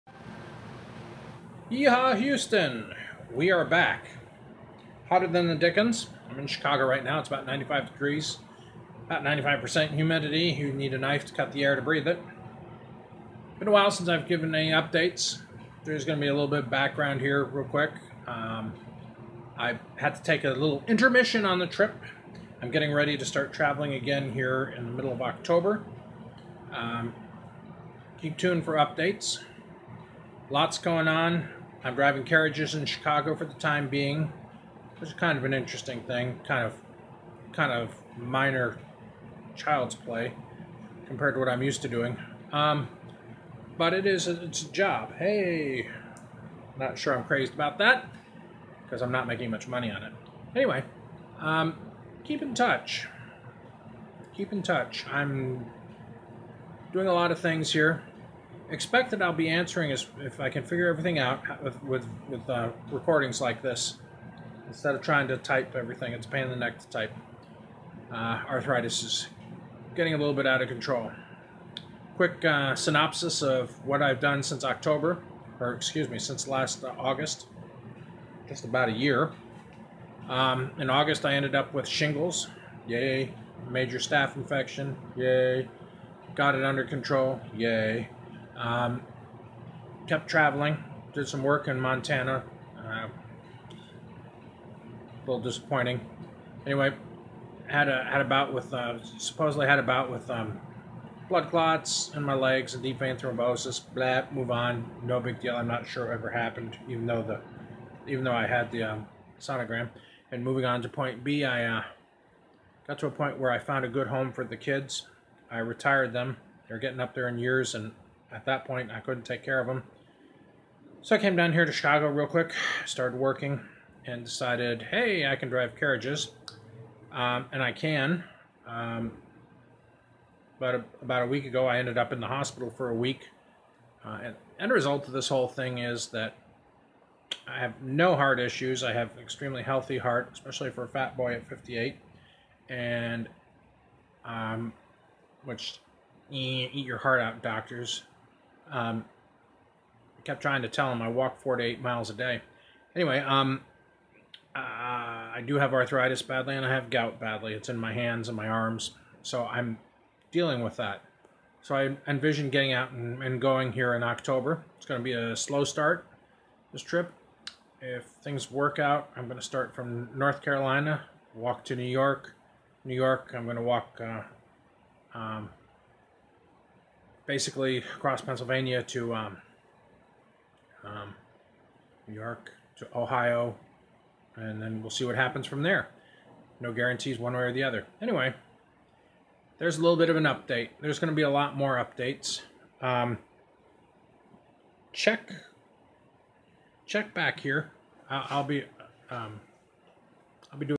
Update 7/9/19
A quick update on what I have been doing and experiencing!

Many more videos to come as I share my journey with all of you, I am happy to be working with horses again in Chicago.